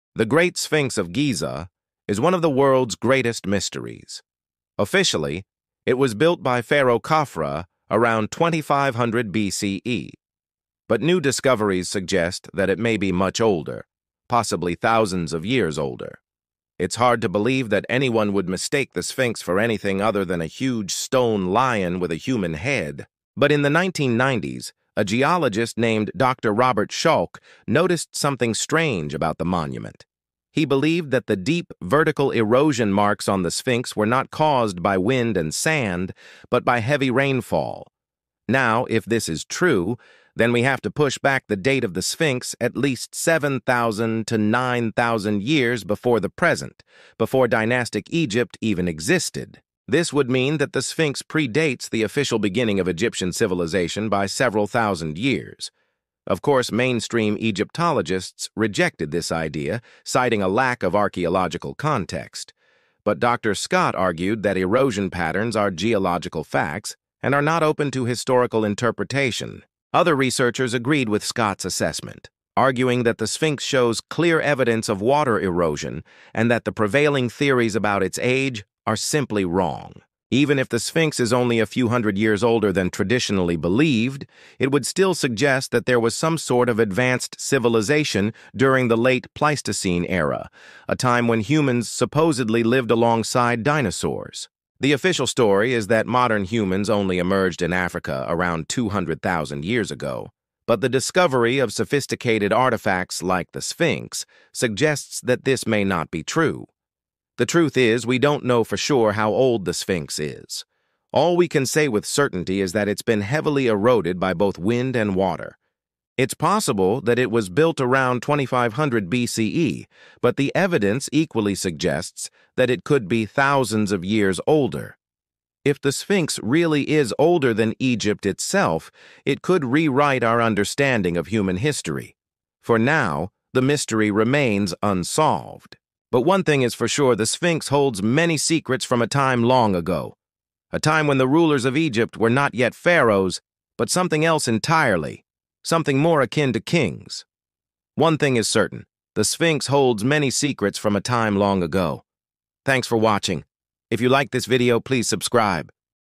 The Sphinx – Is It Older Than Egypt Itself ?
Some geologists say the Sphinx shows signs of water erosion—suggesting it’s thousands of years older than dynastic Egypt. Could this monument predate history as we know it?